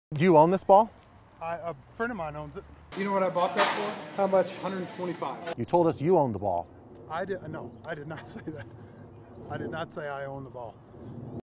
The coach in tonight's invest...
The coach in tonight's investigation says he did nothing wrong. We had our cameras rolling. The story airs at 9 and 10 p.m.